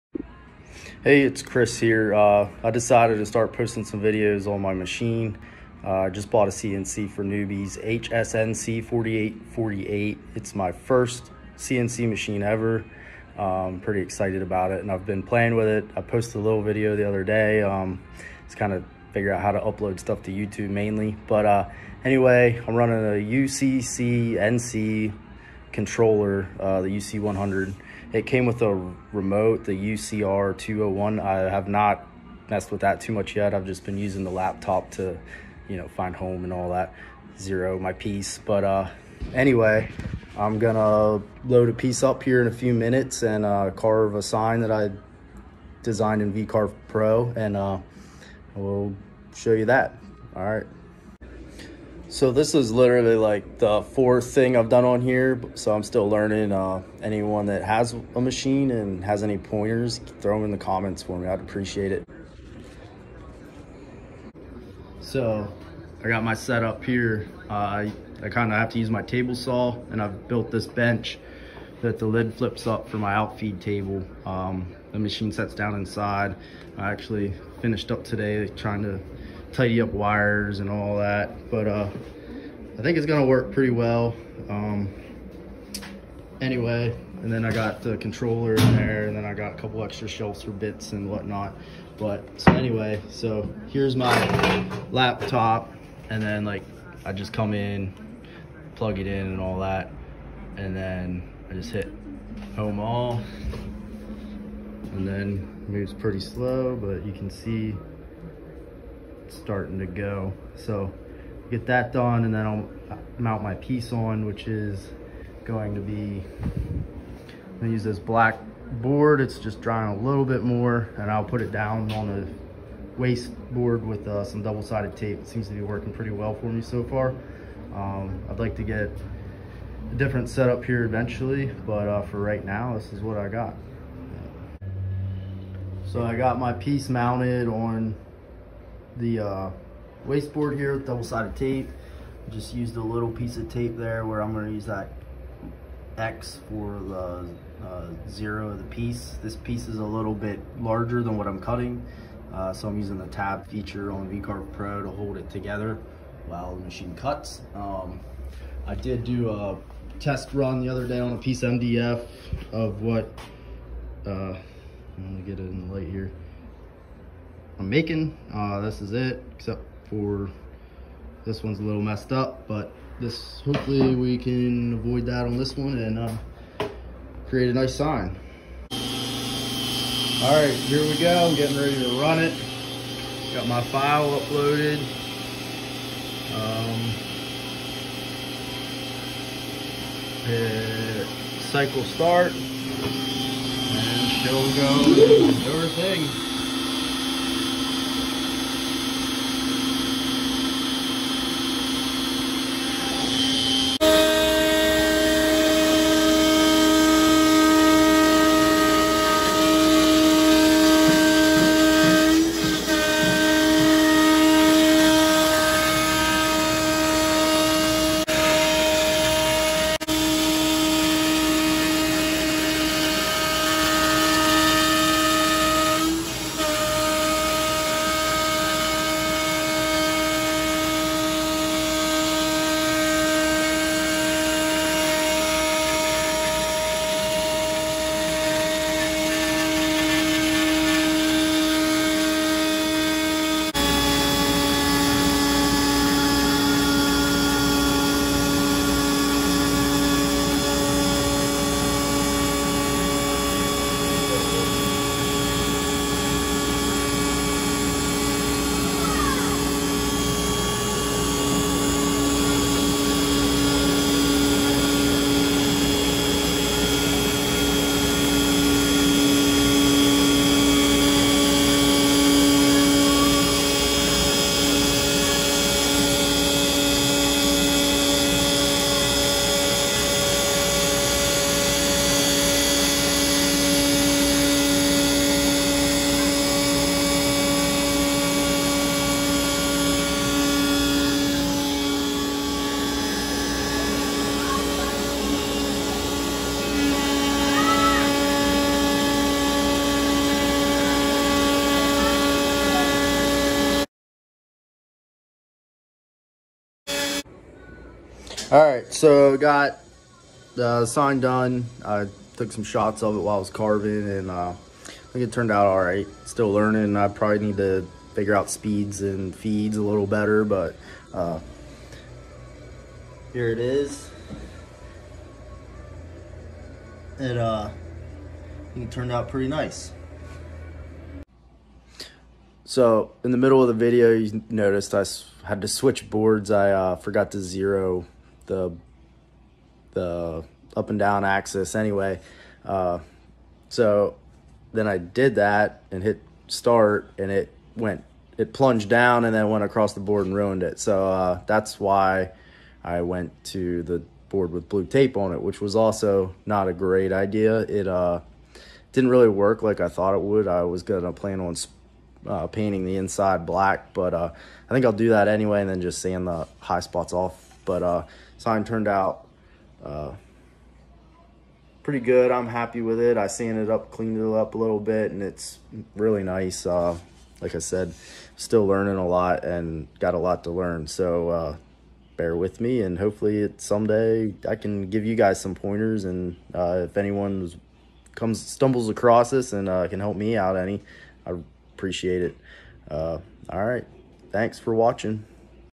CNC sign carve with CNC4Newbie HSNC4848 and UNCC Controller
Just getting started with CNC work and video editing. I learned a lot today about using the CNC and making/editing videos. Next video and carve will be better!